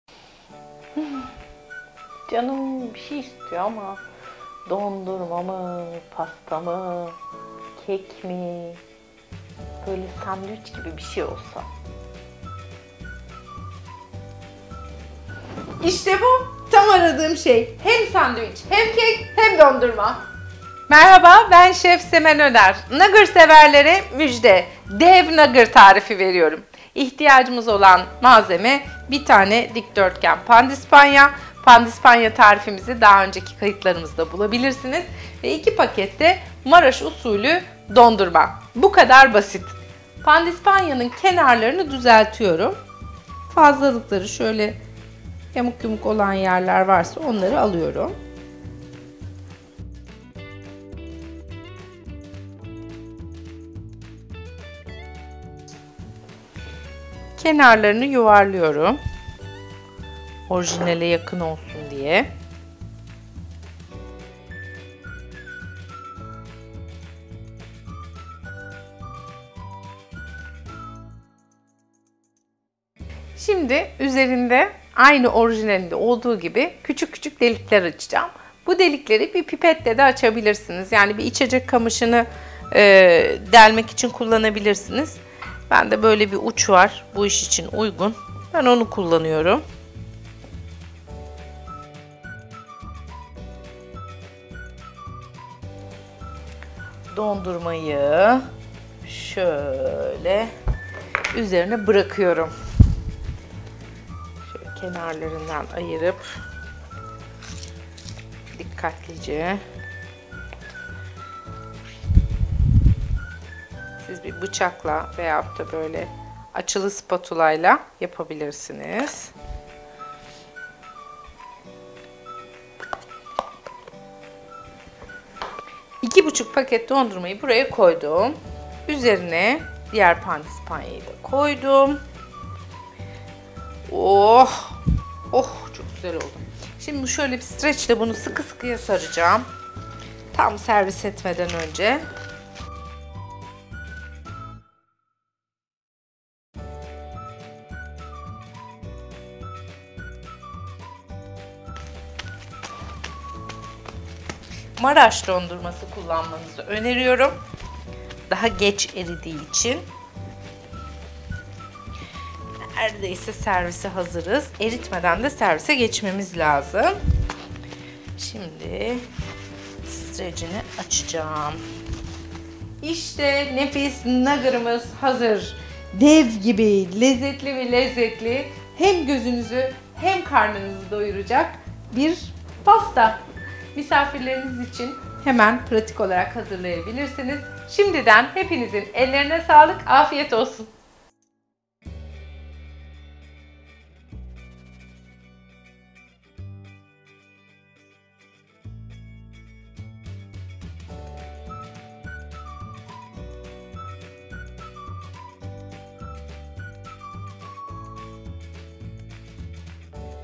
Dev Nogger Sandwich Dondurmalı Pasta - SemenOner - Yemek Tarifleri
Nogger Sandwich Dondurmalı Pasta

Yapılışı
Bir adet dikdörtgen pandispanyanın kenarlarını keserek düzeltin. Köşelerini bir kase yardımıyla yuvarlak şekilde kesin ardından bir pipet yardımıyla pandispanyada küçük delikler açın. Üzerine 2 buçuk paket Maraş dondurması koyun. Üzerini pandispanyanın diğer yarısıyla kapatıp sandviç şekline getirin ve streç filmle sarın. Streç film üzerinden elinizle şekil verdikten sonra, filmi açıp keserek servis yapın.